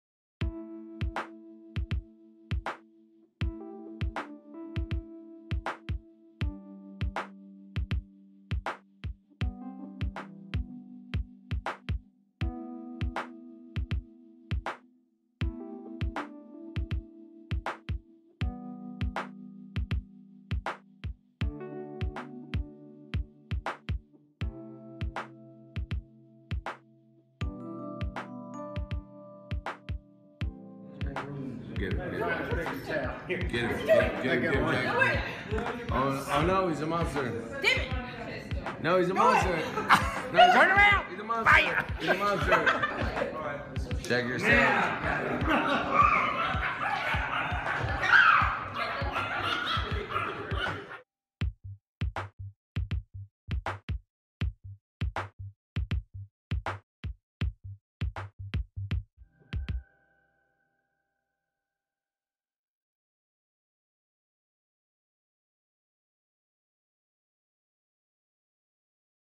Amber Heard, Johnny Depp attended Thanksgiving party with Marilyn Manson | Page Six Celebrity News
Amber Heard and Johnny Depp’s family celebrated with Marilyn Manson at a Thanksgiving bash, new footage that emerged in court Wednesday shows — as the actress was accused of “making up” claims that Depp violently assaulted her.

The British court presiding over Depp’s blockbuster libel trial against The Sun was played footage from the Nov. 26, 2015, gathering in Los Angeles, where Manson jokes with Depp’s son, Jack, 18, about a “monster” being in the room while Heard’s father, David, flicks a scarf around.

“He’s a monster,” the male voice, identified by Heard as Manson, says off-camera.

Heard’s father then takes off chasing Jack around the party, where the actress and Depp were also joined by friend Raquel Pennington.